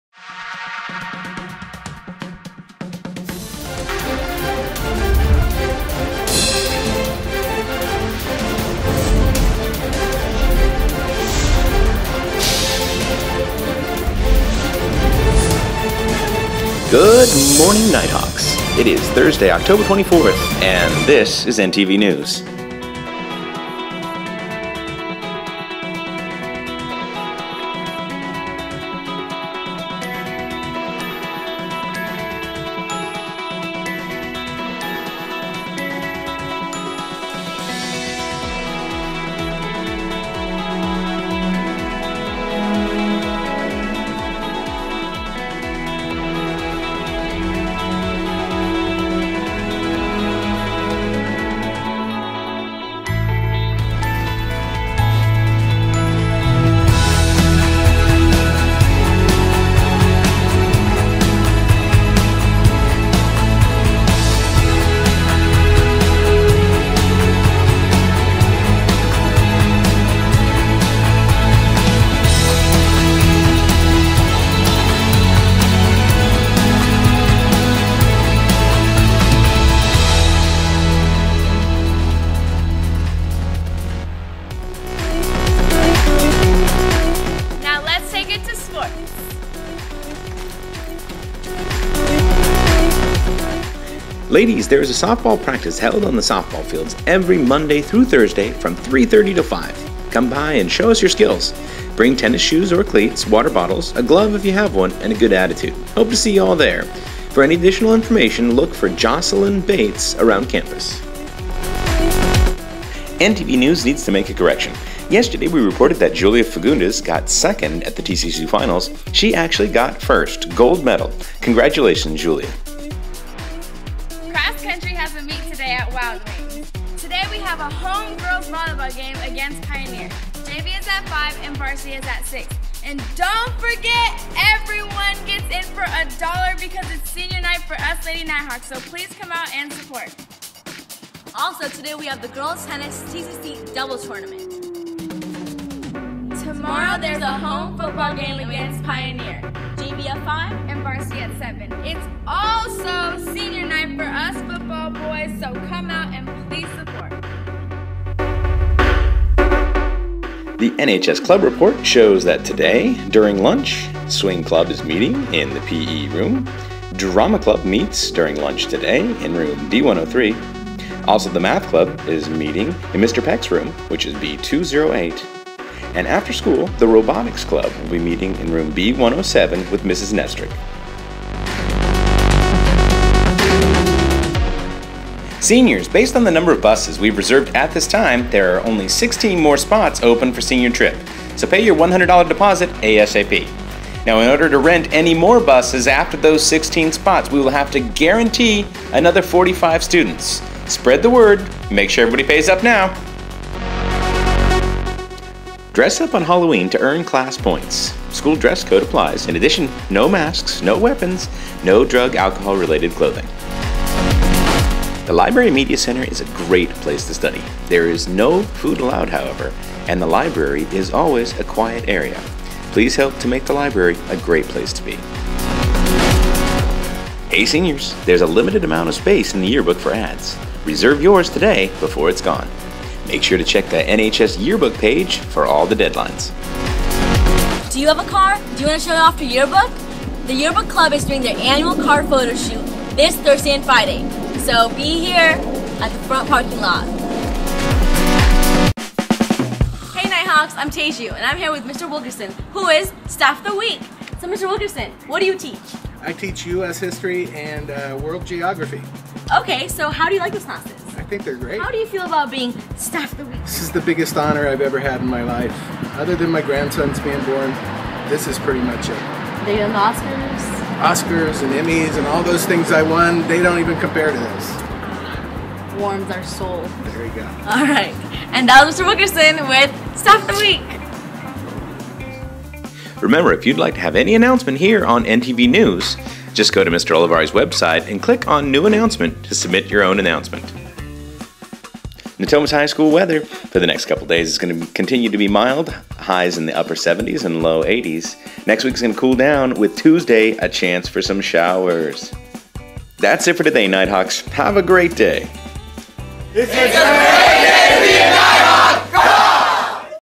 10/24/13 NTV Announcements_
Natomas High School's Morning Announcements produced by NTV.